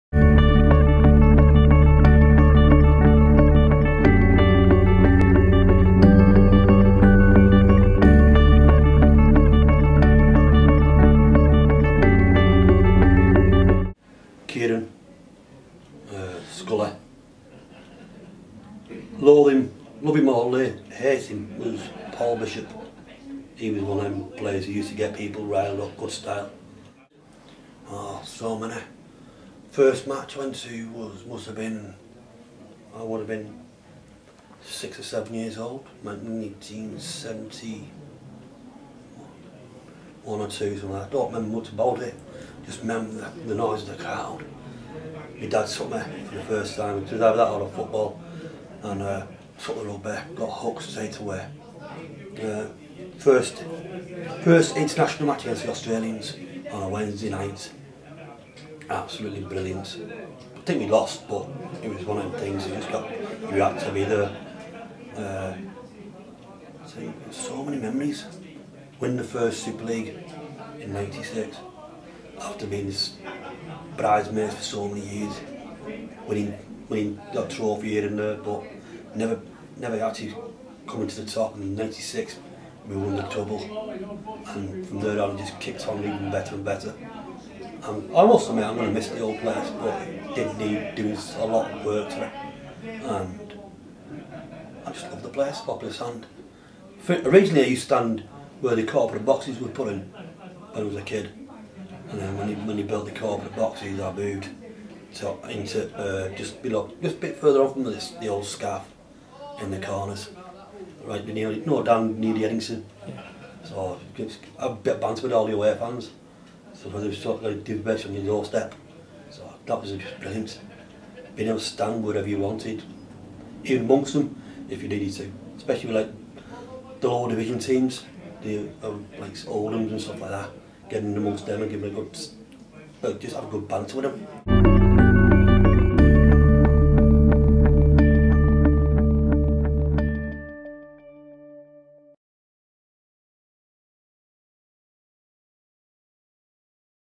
KnowsleyRoad.com Blast17
John remembers the glory in 1996 at Knowsley Road.